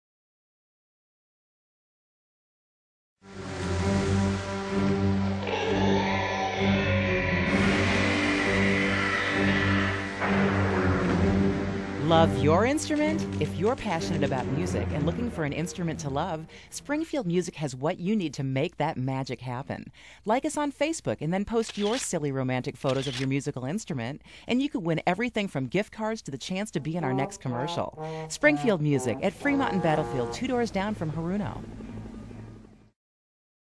Springfield Music Love Your Instrument Spot 1
The first commercial in the Love Your Instrument Campaign for Springfield Music